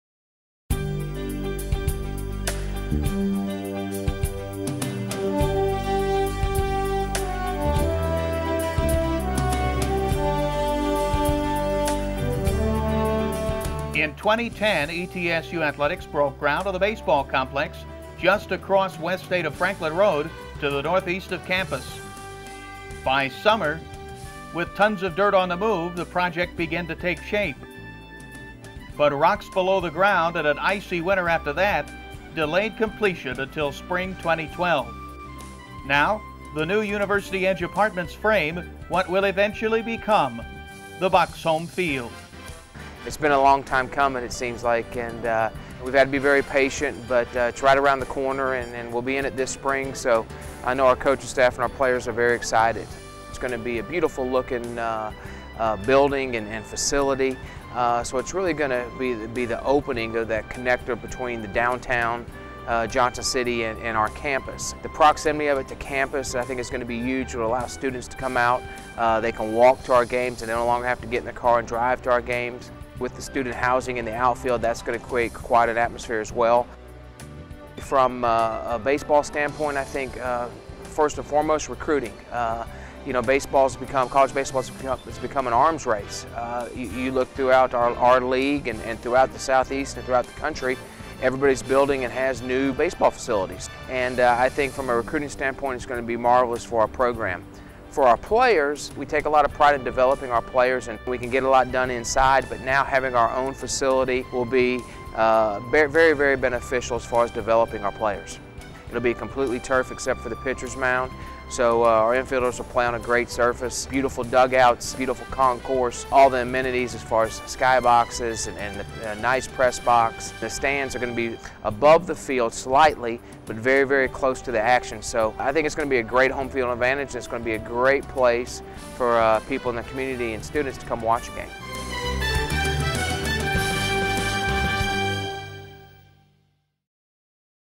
Facilities - Baseball Complex
ETSU Baseball Complex.  Scheduled completion: Spring 2012.